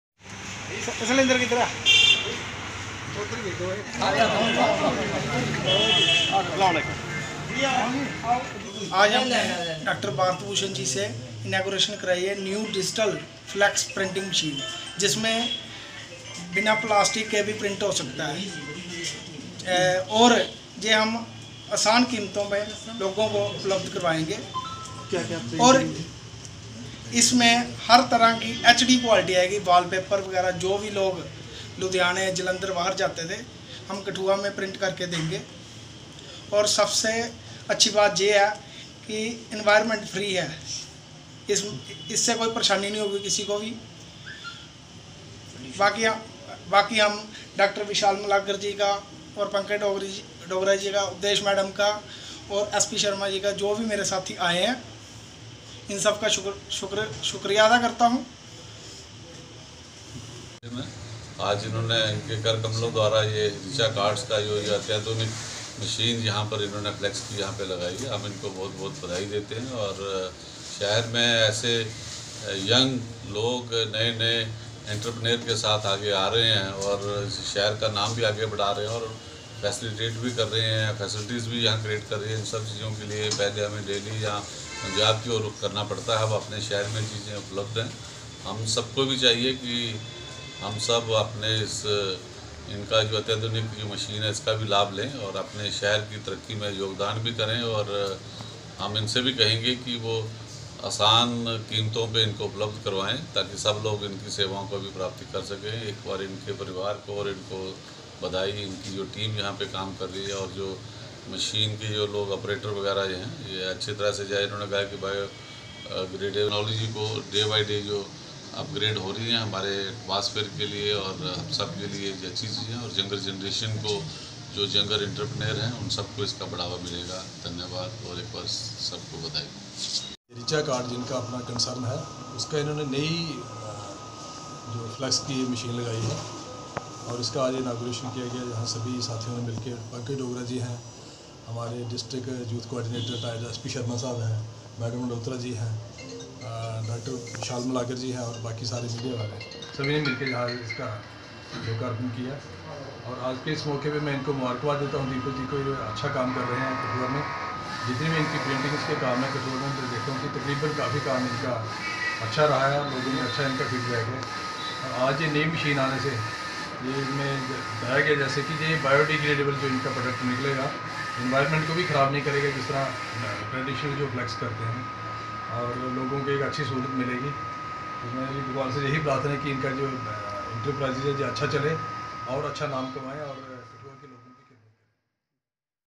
कठुआ में डिजिटल फ्लेक्स मशीन का उद्घाटन डॉक्टर भारत भूषण ने किया
कठुआ में डिजिटल फ्लेक्स मशीन का उद्घाटन डॉक्टर भारत भूषण ने किया रिचा प्रिंटिंग प्रेस के मालिक दीपक गुप्ता ने पठानिया मार्किट कठुआ में डिजिटल फ्लेक्स मशीन लगाई।